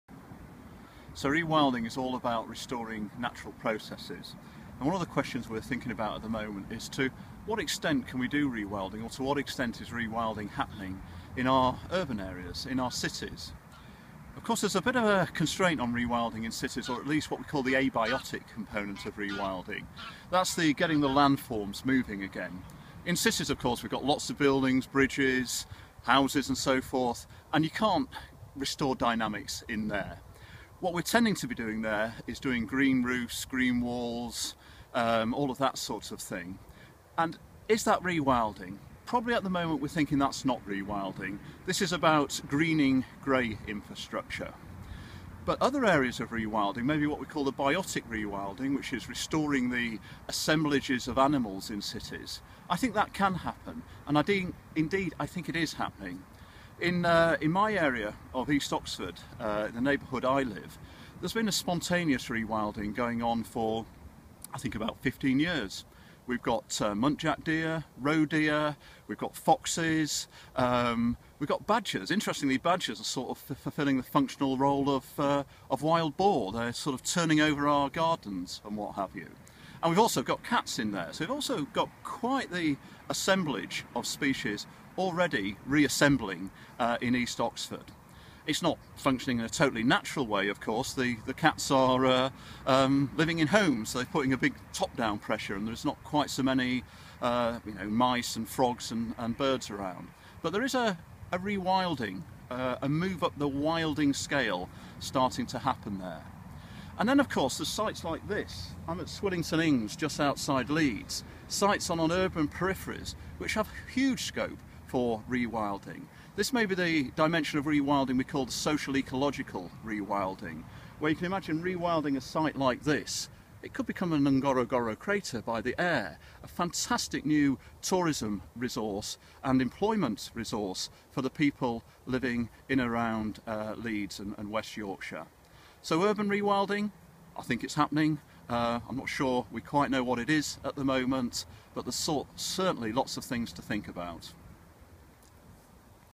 Reflections on urban rewilding
As interest in rewilding grows it is important to position the concept in relation to urban conservation. Here are a couple of my thoughts on the topic.